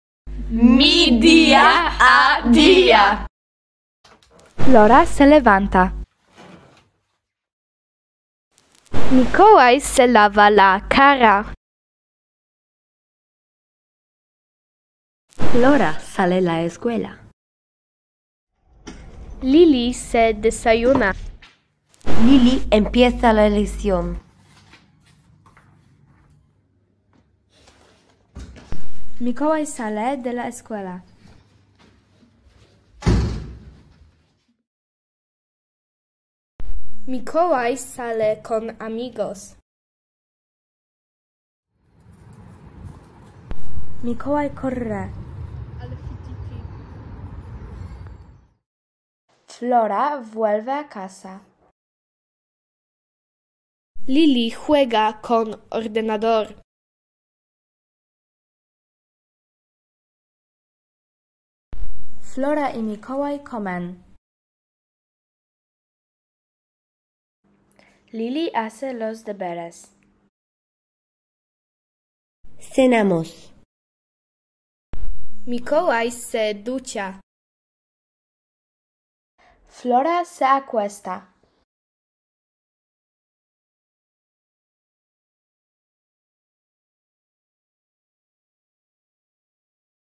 Mi día a día - czyli dzień jak co dzień po hiszpansku w wykonaniu uczniów i uczennic klas 7.
Codzienne czynności - czyli projekt filmowy na zaliczenie działu w podręczniku z odmianą czasowników.